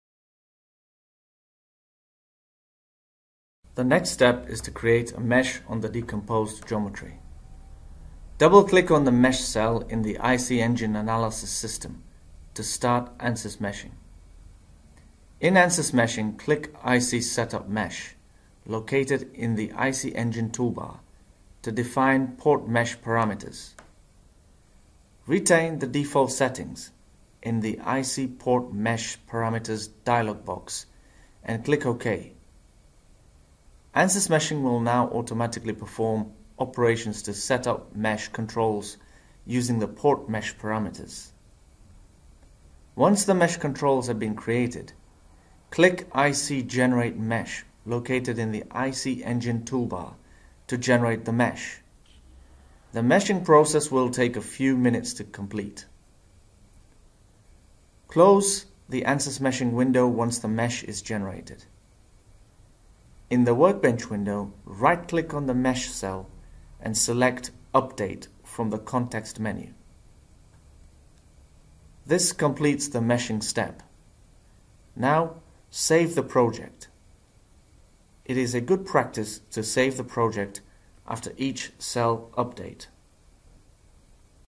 ANSYS Internal Combustion Engine (ICE): Port Flow Part 3 - Meshing
This 6-part tutorial of ANSYS How To videos will demonstrate the setup and port flow simulation of an internal combustion engine in ANSYS Internal Comubstion Engine (ICE). Part 3 of 6.